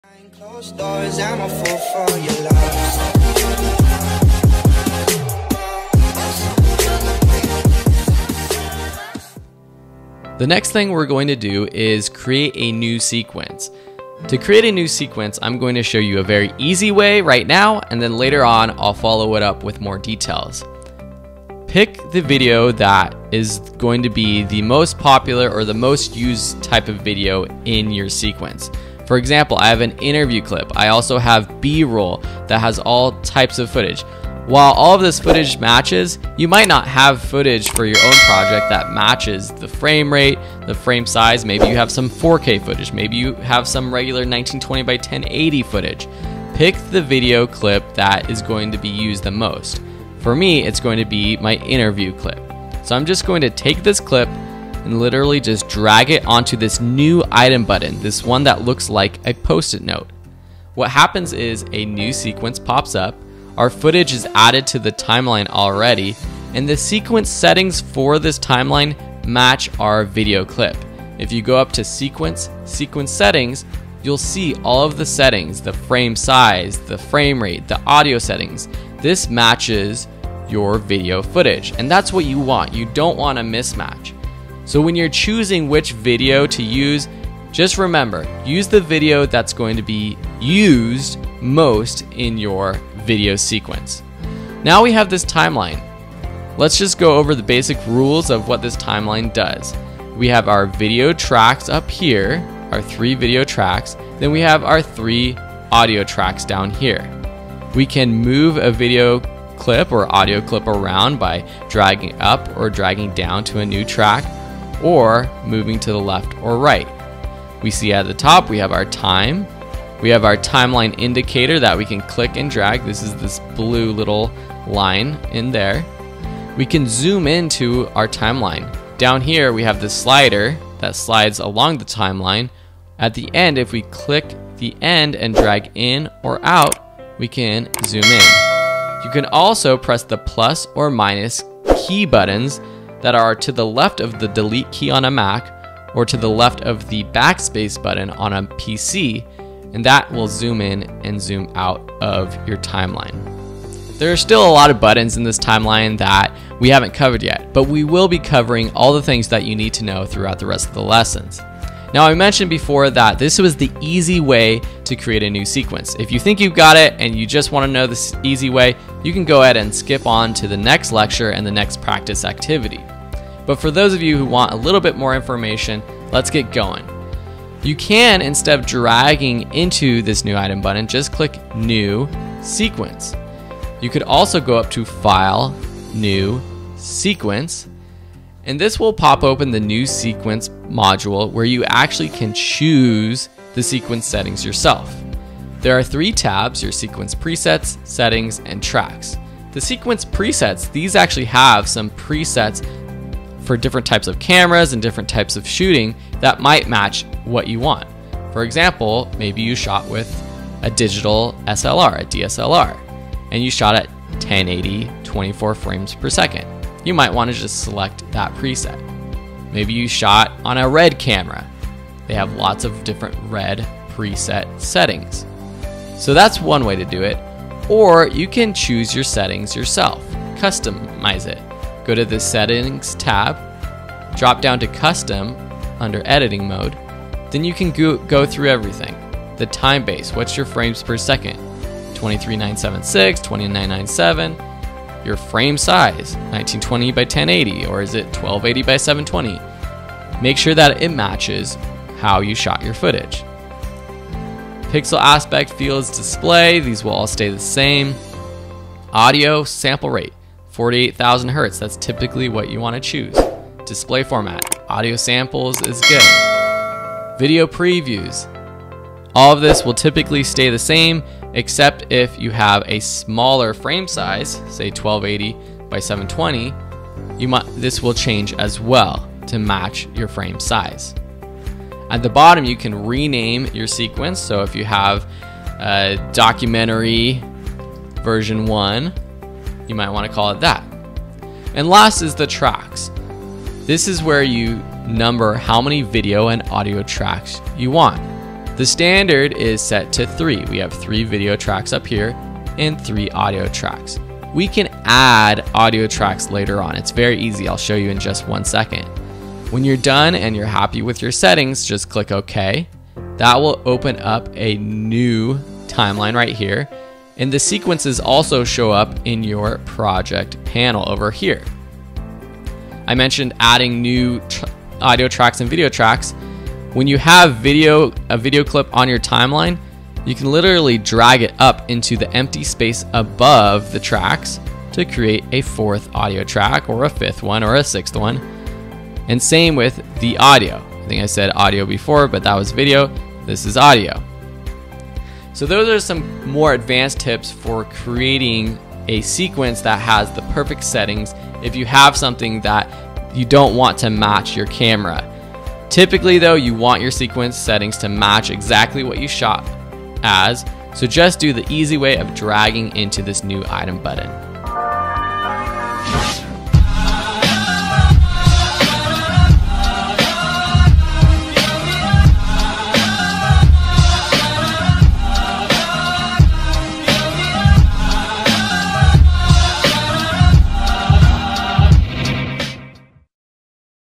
Part 5 : Create a New Sequence and new Timeline - Adobe Premiere Pro CC
Hello Everyone!
Here's Part 5 of AdobePremierePro Class. In this video you will learn how to create a newsequence in Adobe Premiere Pro CC and some details about the Timeline window.